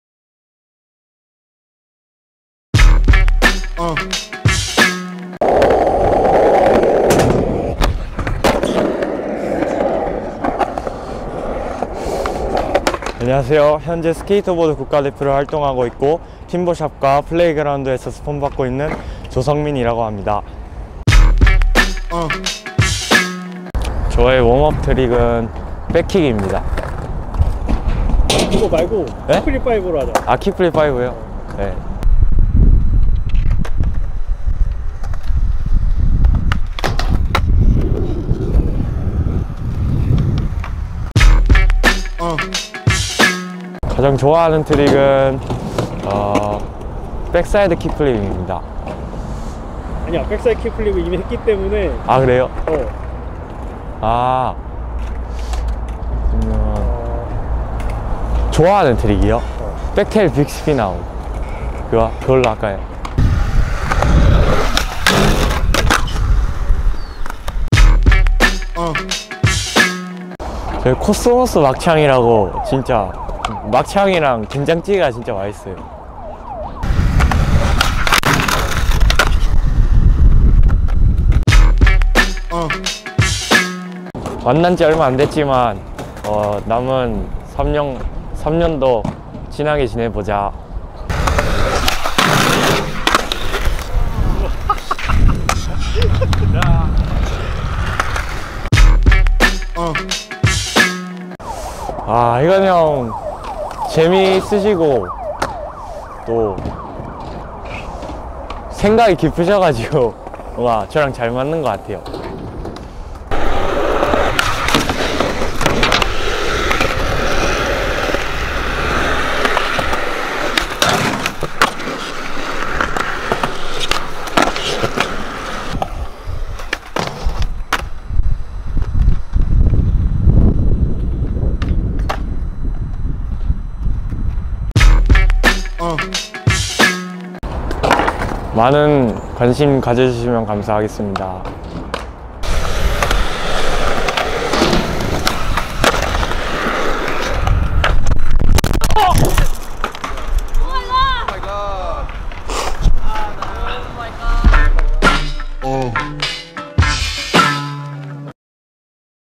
FIVE 4 FIVE: 조성민 (Sungmin Cho)
평소에는 댕댕미를, 스케이트보드 위에선 누구보다 성숙한 스타일을 보여주는 팀버샵과 국가대표 소속 라이더, 조성민을 소개한다.

얼마 전, 난 국가대표팀 훈련이 뚝섬으로 잡혔다는 소식을 들었다. 이것은 평소에 만나기 어려운(대구에 살고, 평소 전지훈련을 나가기 때문) 성민이를 만날 수 있는 좋은 기회! 난 부랴부랴 성민이에게 연락하여 약속을 잡았다.

성민이는 역시나 믿음직스러운 실력을 보여줬다. 다만, 촬영하던 도중 필르머(본인)의 심한 부상 및 카메라 개박살로 인해 촬영을 더 이상 이어갈 수 없어서 매우 아쉬울 따름. 그래서 직접 물어봤다. 한국 축구 팬들에게 전하고 싶은 이야기를.

안 본 사람은 있어도 한 번만 본 사람은 없을, 조성민의 FIVE 4 FIVE를 만나보자.

* 이 영상은 팀버샵(Timber Shop)의 후원을 받아 제작되었습니다.